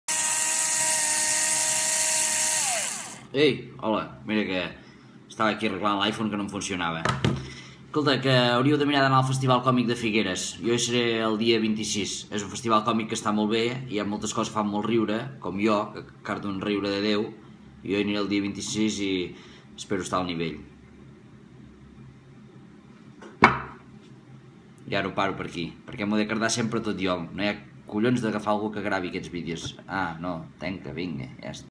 En Peyu et convida al FestivalCòmic -,Festival Còmic 2016
En Peyu, tot i que el veieu així de seriot és un humorista i molt particular! Et pots cardar un fart de riure amb ell perquè quan arrenca no hi ha qui el pari! Ell també participarà en la Nit de Micro de dissabte 25 al GestivalCòmic de Figueres